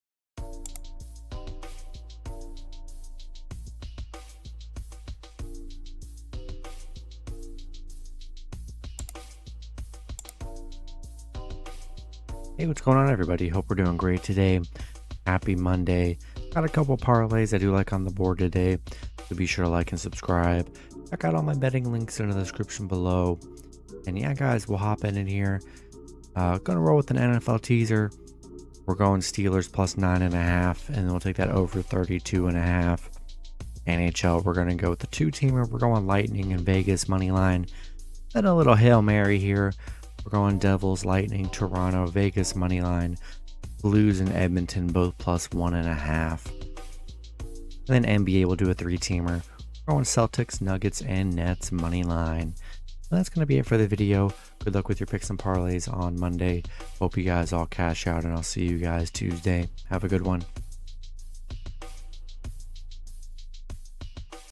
Free Multi-Sport Parlay Options for today 11/28/22
Here are my Free Multi sport parlay options for today

🏆Top Sportsbooks I use:
2. BOVADA
3. My Bookie

Music I use in my videos:
1. Music By The Relaxed Movement

✔️Parlays for today:
Teaser
Steelers +9.5
Over 32.5

NHL
Lightning ML
Vegas ML

Devils ML
Lightning ML
Toronto ML
Vegas ML
Blues +1.5
Edmonton +1.5

NBA
Celtics ML
Nuggets ML
Nets ML